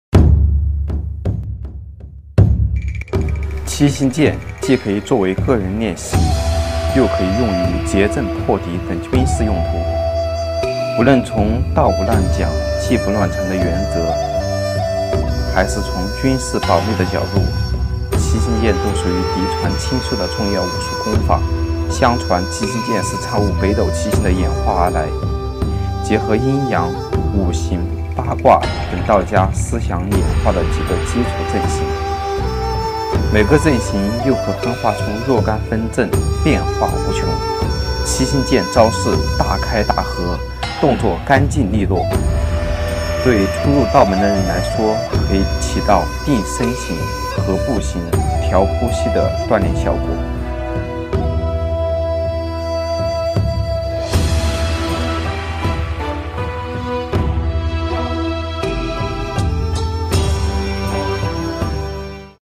第1集：武当剑基本功，介绍
参悟北斗七星的演化而来的七星剑，既可以摆出战斗队形，又可以让初学者得到有效锻炼。天人和一，合众为一！
摊牌了，我们不装了，我们中国人会功夫。功夫
If you want learn kungfu, you can follow.